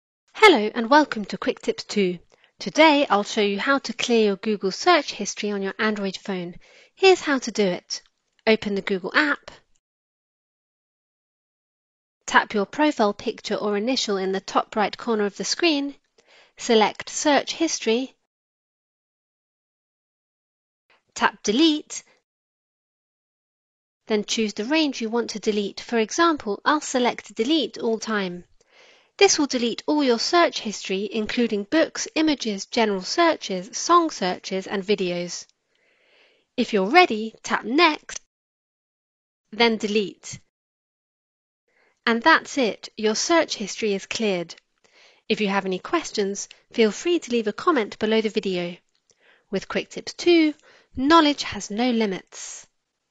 How to Clear Google Search History on Android phone
Today, I’ll show you how to clear your Google search history on your Android phone. Here’s how to do it:

Open the Google app.
Tap your profile picture or initial in the top right corner of the screen.
Select Search history.
Tap Delete, then choose the range you want to delete. For example, I’ll select Delete all time.
This will delete all your search history, including books, images, general searches, song searches, and videos.

If you're ready, tap Next, then Delete.

And that’s it—your search history is cleared!